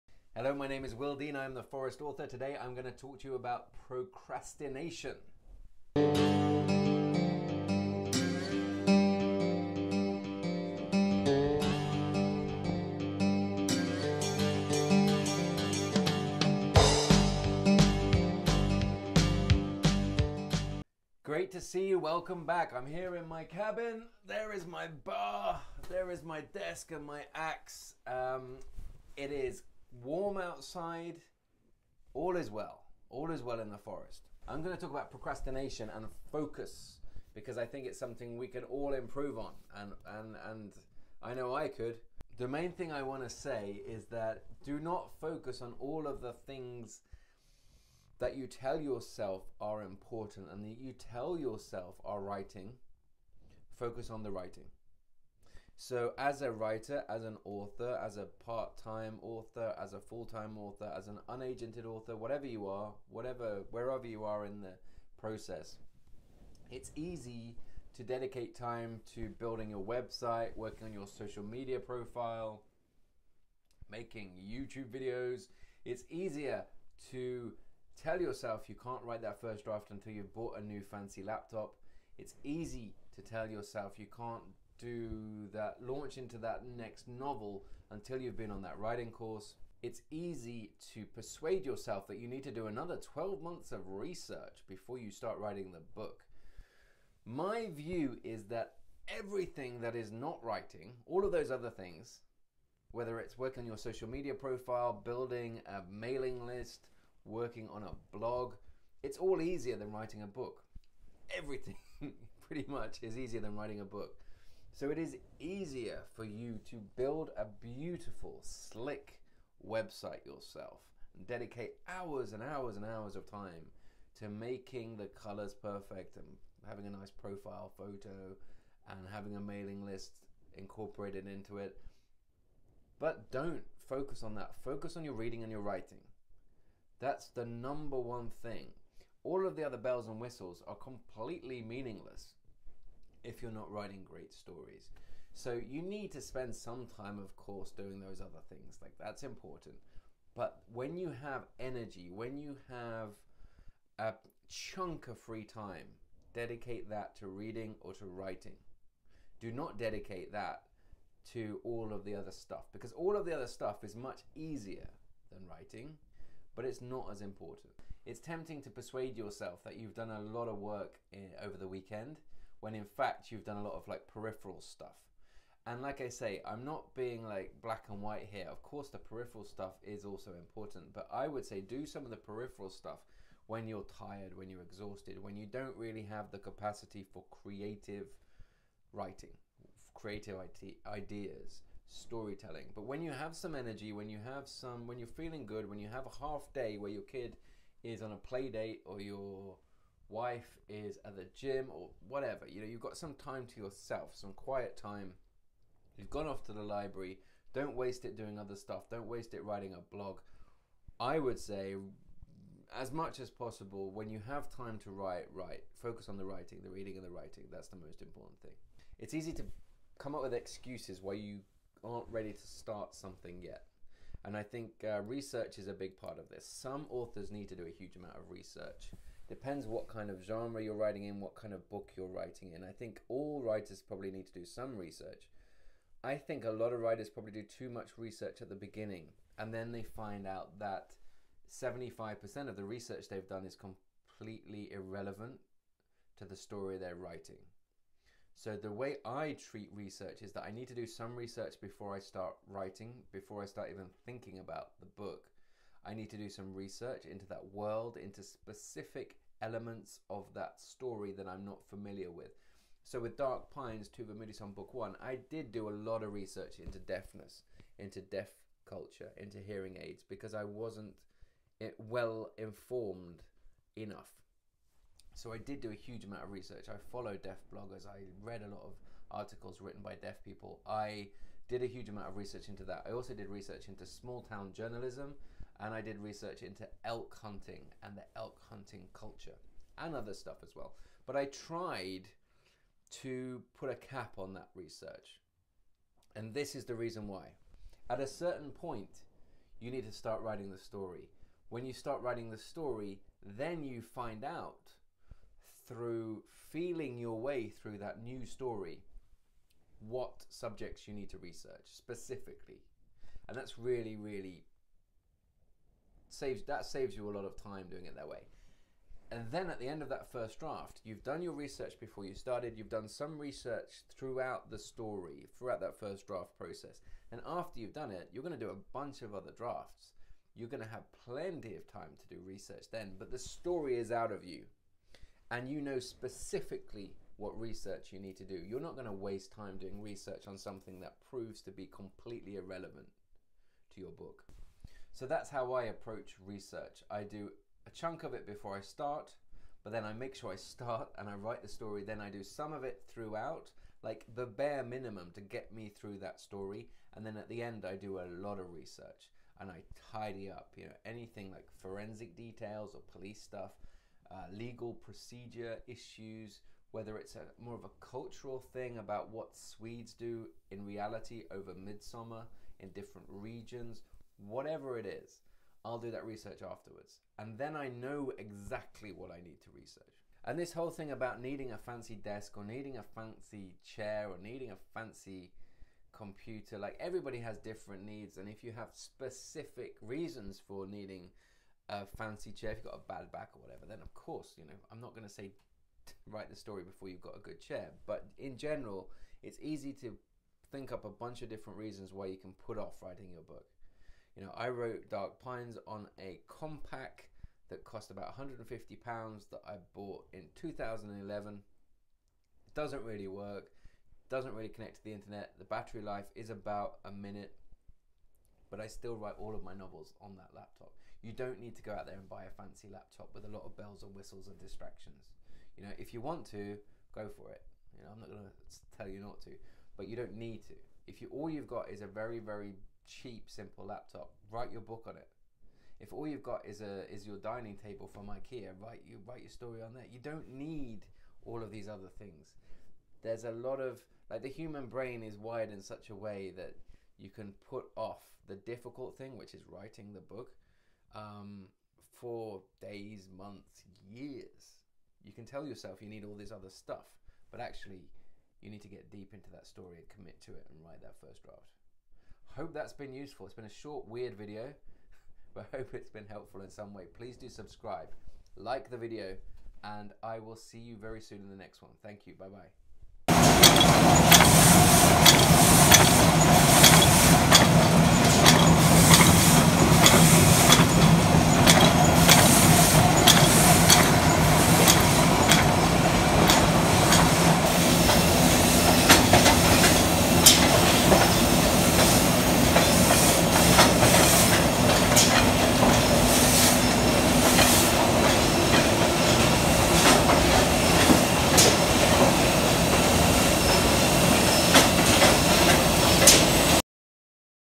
Procrastination (author websites, social media, laptops, blogs, research, protecting writing time)
We all do it.

Procrastination.

Starting a new project is always daunting. It’s easy to delay and tell yourself you need a better website/better computer/ more research. But at some point you have to leap into the void. You have to start actually writing.

In this video I discuss different approaches to research. I talk about priorities and effective time management.

W

All music/FX used under licence (paid for) via StoryBlocks for LumaFusion.